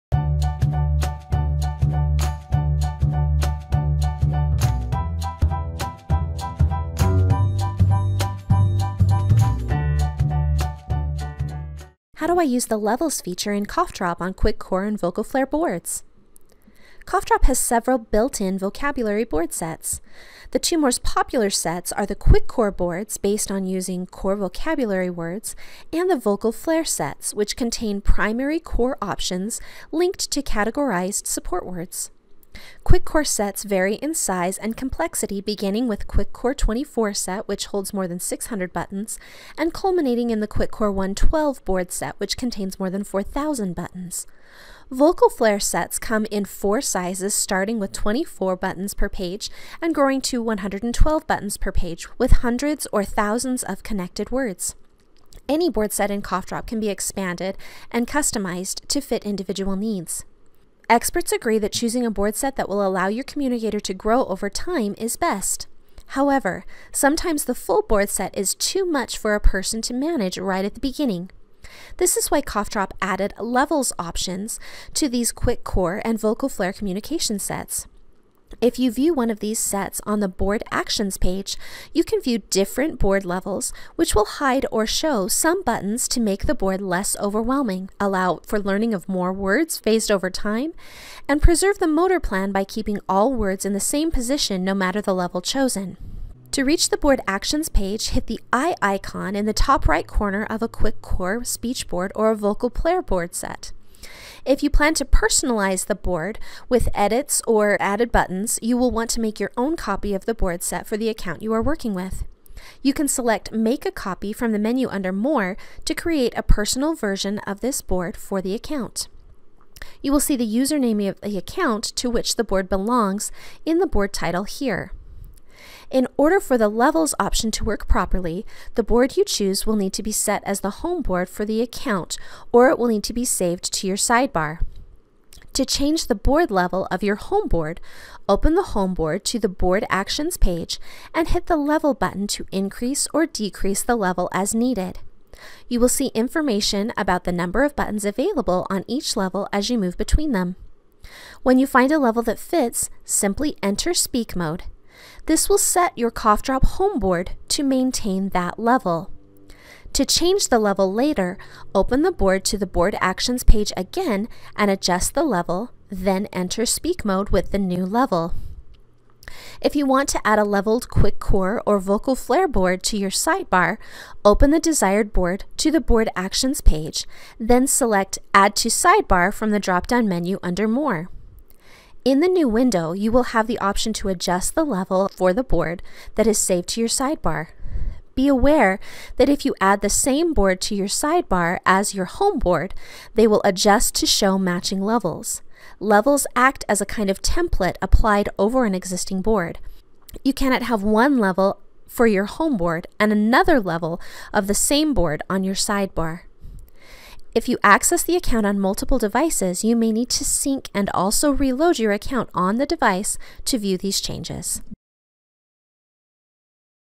CoughDrop board levels for AAC
Changing board levels for Quick Core and Vocal Flair boars within CoughDrop for AAC